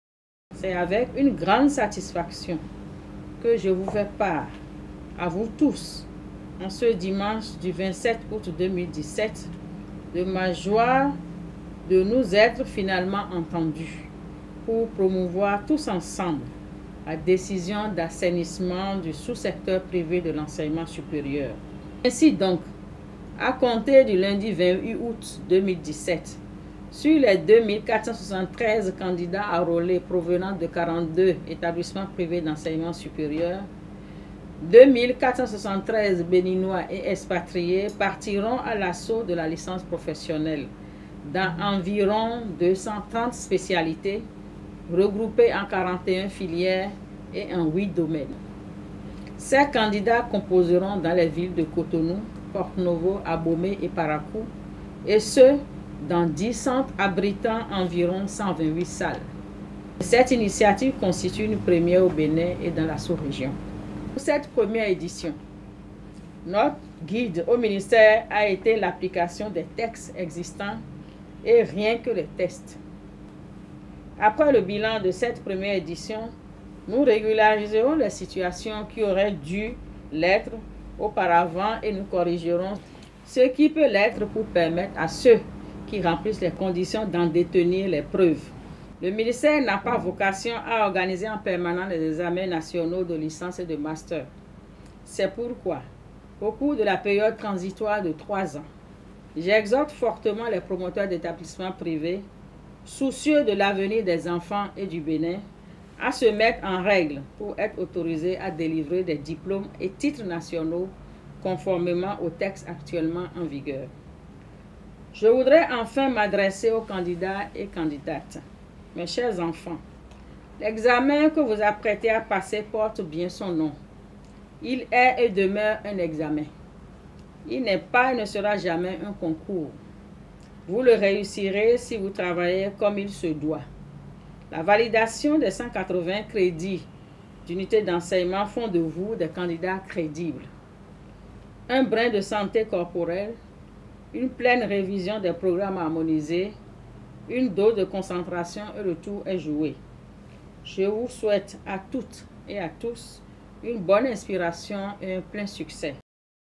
Examens nationaux de Licence et de Master : le message de la ministre de l'enseignement supérieur
A la veille des examens nationaux de licence et de master, la ministre Marie-Odile Attanasso de l'enseignement supérieur s'est adressé aux candidats et autres acteurs concernés. Voici l'intégralité de son message.